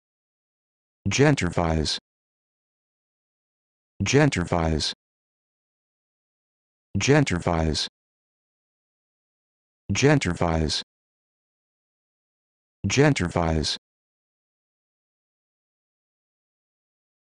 How to Pronounce Gentrifies
This video shows you how to pronounce Gentrifies